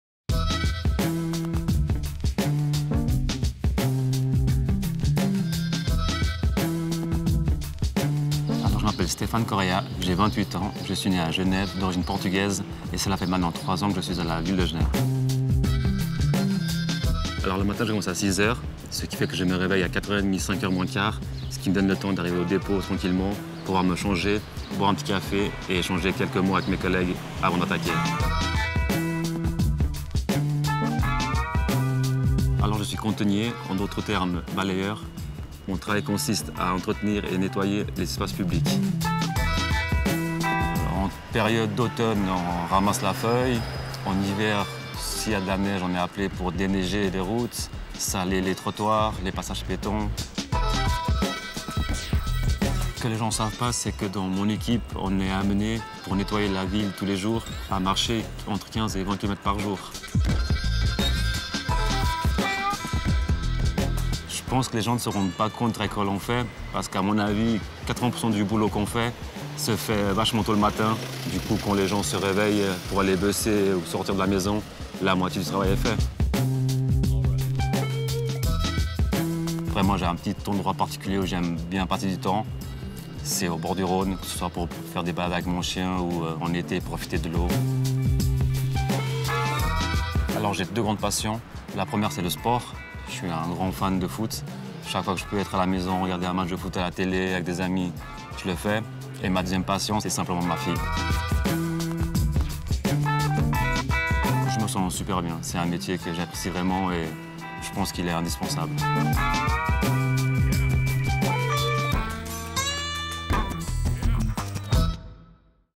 Portrait de Stéphane Correia - Voirie de la Ville de Genève
Stéphane Correia nous raconte ses journées au service voirie de la Ville de Genève. Malgré la crise, le service poursuit son travail tous les jours de l'année. L'occasion de mettre en lumière le travail de ces hommes et femmes qui oeuvrent au quotidien pour maintenir la ville aussi propre que possible!